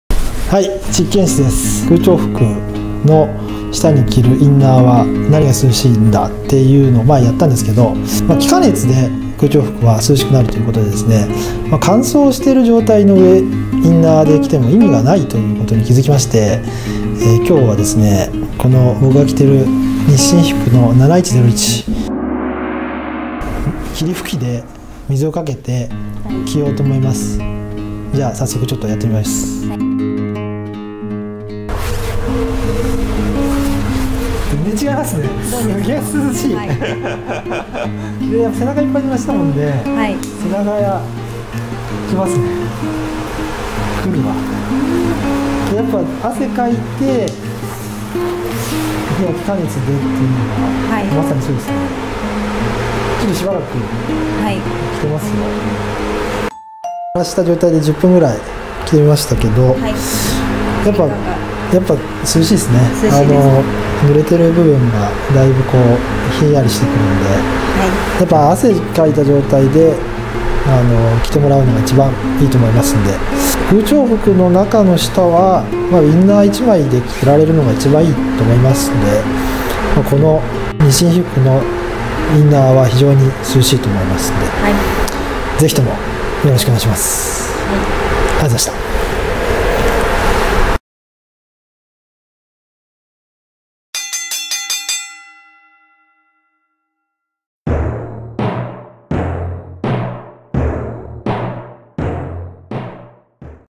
実験室。空調服の下に濡らしたコンプレッションインナーを着てみました！
気化熱で涼しくなるという空調服。下着が濡れてなきゃダメでしょ！ってことで、濡らして着てみました。実際にどのように感じるのか！？

this is 2015year new model.We wear Air conditioning suts.
Air conditioning Blouson.It's cool.Sporty Blouson
Japan quality